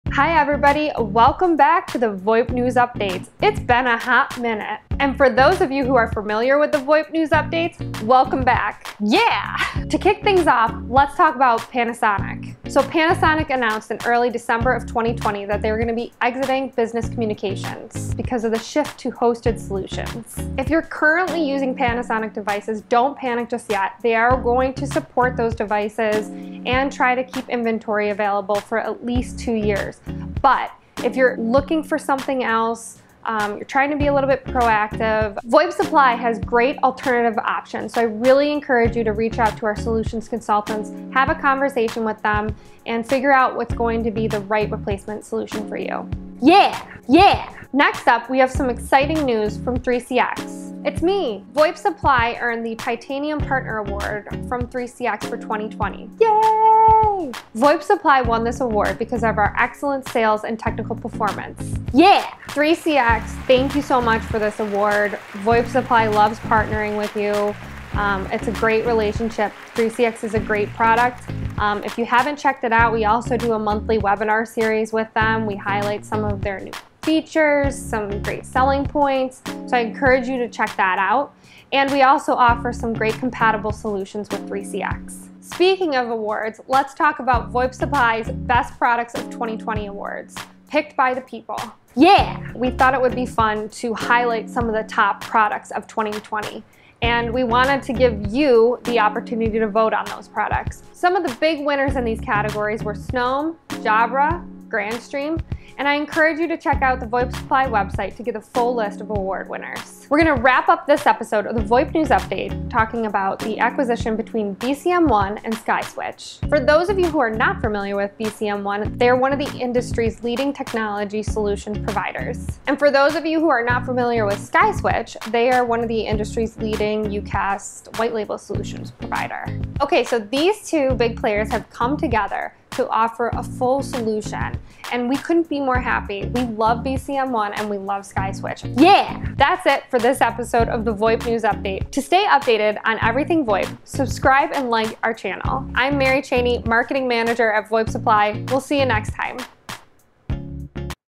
VoIP News Update | February 2021 - Panasonic, Bcm One, and more!
Start off the new year by getting all of your VoIP industry news in one place! Welcome back to the VoIP News Update!

In this episode, we'll cover Panasonic's exit from the business communications market, the acquisition between BCM One and Sky Switch, 3CX, and more!

We're everything you need for VoIP!

Mary Cheney:
Hi, everybody. Welcome back to the VoIP News Updates. It's been a hot minute. And for those of you who are familiar with the VoIP News Updates, welcome back.
Mary Cheney:
Yeah. To kick things off, let's talk about Panasonic. So Panasonic announced in early December of 2020 that they were going to be exiting business communications because of the shift to hosted solutions. If you're currently using Panasonic devices, don't panic just yet. They are going to support those devices and try to keep inventory available for at least two years.
Mary Cheney:
But if you're looking for something else, you're trying to be a little bit proactive, VoIP Supply has great alternative options. So I really encourage you to reach out to our solutions consultants, have a conversation with them, and figure out what's going to be the right replacement solution for you. Yeah.
Mary Cheney:
Yeah. Next up we have some exciting news from 3CX. It's me! VoIP Supply earned the Titanium Partner Award from 3CX for 2020. Yay! VoIP Supply won this award because of our excellent sales and technical performance.
Mary Cheney:
Yeah. 3CX, thank you so much for this award. VoIP Supply loves partnering with you. It's a great relationship. 3CX is a great product. If you haven't checked it out, we also do a monthly webinar series with them. We highlight some of their new features, some great selling points, so I encourage you to check that out, and we also offer some great compatible solutions with 3CX.
Mary Cheney:
Speaking of awards, let's talk about VoIP Supply's Best Products of 2020 Awards, picked by the people. Yeah. We thought it would be fun to highlight some of the top products of 2020, and we wanted to give you the opportunity to vote on those products. Some of the big winners in these categories were Snom, Jabra, Grandstream. And I encourage you to check out the VoIP Supply website to get a full list of award winners.
Mary Cheney:
We're going to wrap up this episode of the VoIP News Update talking about the acquisition between BCM One and SkySwitch. For those of you who are not familiar with BCM One, they're one of the industry's leading technology solution providers. And for those of you who are not familiar with SkySwitch, they are one of the industry's leading You-Cast white-label solutions provider. Okay, so these two big players have come together to offer a full solution, and we couldn't be more happy. We love BCM One and we love SkySwitch.
Mary Cheney:
Yeah. That's it for this episode of the VoIP News Update. I'm Mary Cheney, Marketing Manager at VoIP Supply.

Follow VoIP Supply On Social Media!